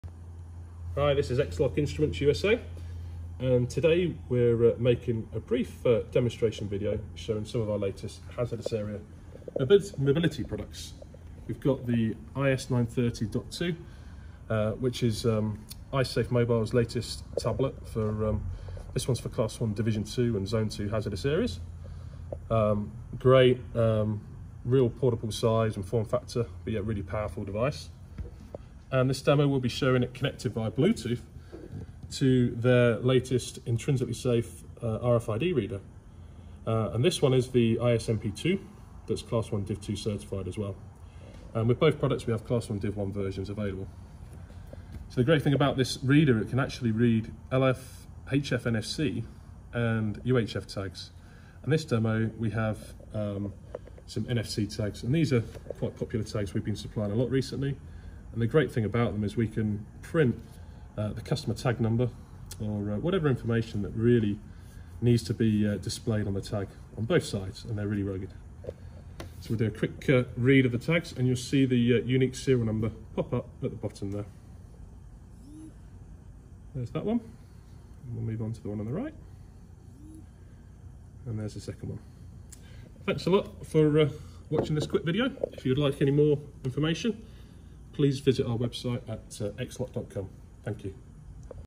Demo of IS930.2 Hazardous Area Certified Android Tablet & IS Safe MP.2 Bluetooth UHC/NFC RFID Reader
Interested in automating your inspections? Check out this short demo video showing one of our great solutions for your maintenance and operations crews featuring the IS930.2, hazardous area certified Android tablet and IS-MP.2 Bluetooth UHF/NFC RFID reader from i.safe MOBILE with our popular printable flex NFC tag. Just add the app of your choice to the tablet and you have a slick, cost effective automated inspection solution for your crews, process industry rated!